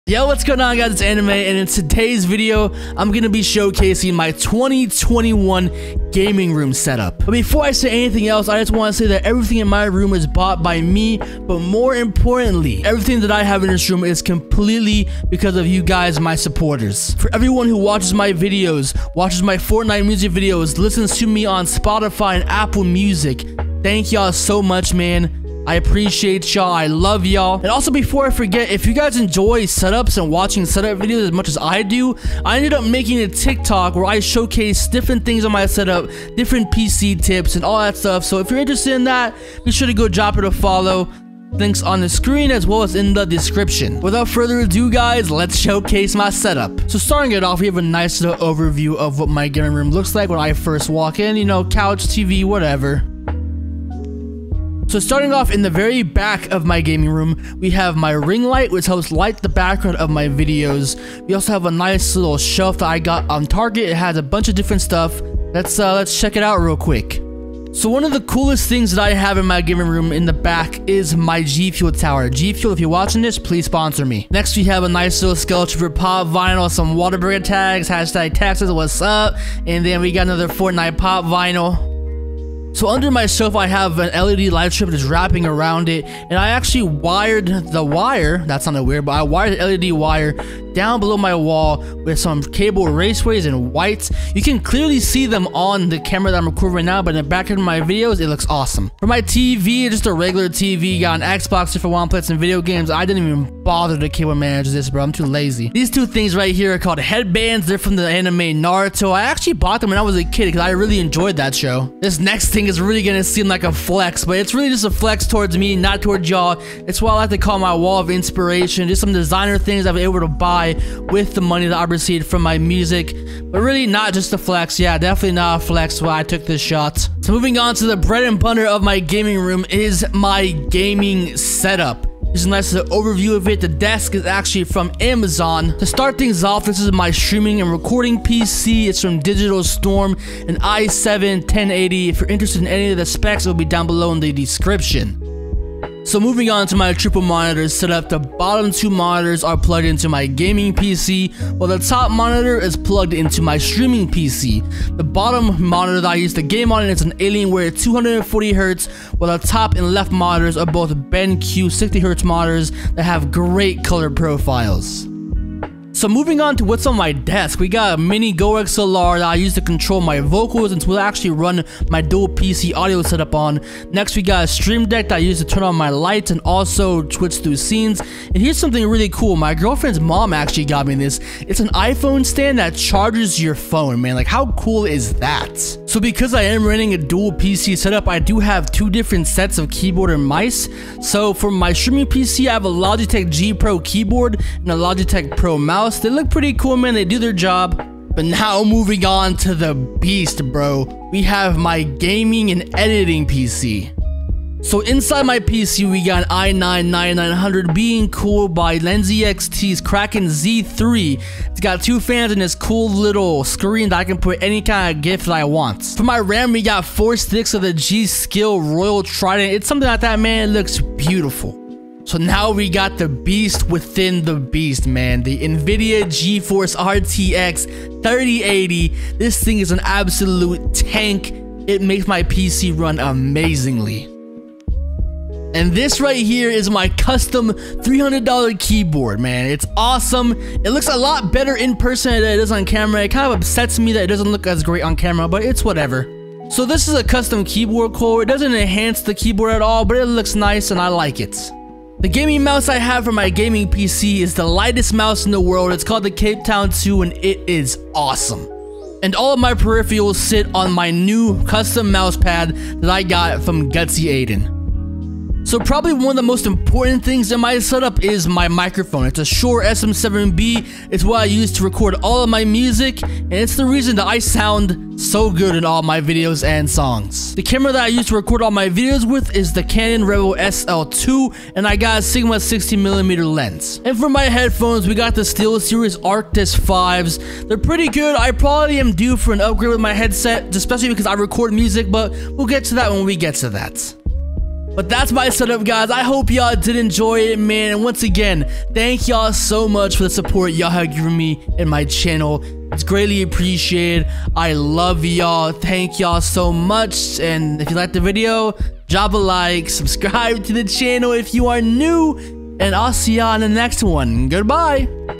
MY 2021 GAMING SETUP TOUR!
-LINKS FOR EVERYTHING IN SETUP-
Alienware Monitor
BenQ Monitor
Elgato Stream Deck
Logitech Keyboard
Logitech Mouse
Phone Charger
Elgato Keylights
Ring Light
LED Light Strips
Philips Hue Lights
Gaming Desk
DSLR Camera
Camera Lens